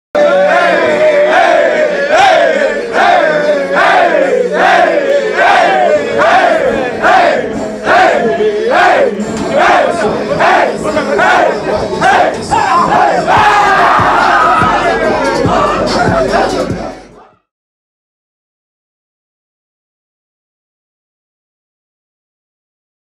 Milton Partied In The Locker Room Before Partying On Alpharetta
Jordan McDonald & Co. didn't seem too worried about Alpharetta during pre-game in the locker room.